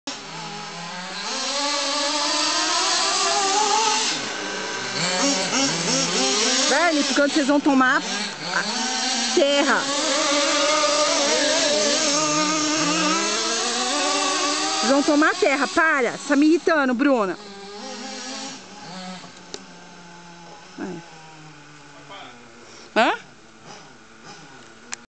Kyosho Inferno Pega da Equipe Toca Racing Pitangueiras Video 3
Equipe Toca Racing de Pitangueiras treinando na pista com dois Kyosho Inferno,sendo um Inferno MP7.5 com motor Novarossi 21 e outro Inferno Sports com motor 21 original